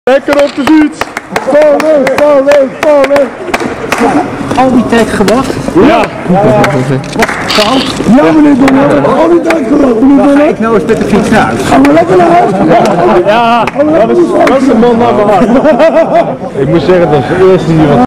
Donner laatste keer met de fiets na val kabinet
Donner neemt afscheid van toeschouwers om vier uur 's nachts.
Na een hete nacht binnen en een koude nacht buiten rolden de kop (stukken) (pen) naar buiten.